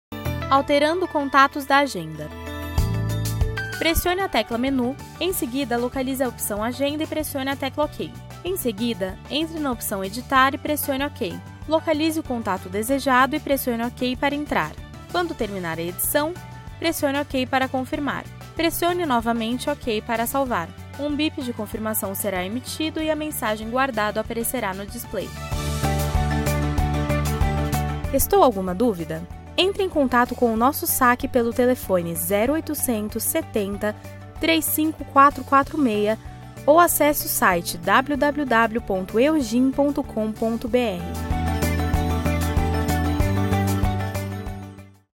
ALTERANDO CONTATOS DA AGENDA
Pressione a tecla “MENU”, em seguida localize a opção AGENDA e pressione a tecla “OK”, Em seguida, entre na opção EDITAR e pressione “OK”. Localize o contato desejado e pressione “OK” para entrar. Quando terminar a edição, pressione “OK” para confirmar. Pressione novamente “OK” para salvar. Um bip de confirmação será emitido e a mensagem GUARDADO aparecerá no display.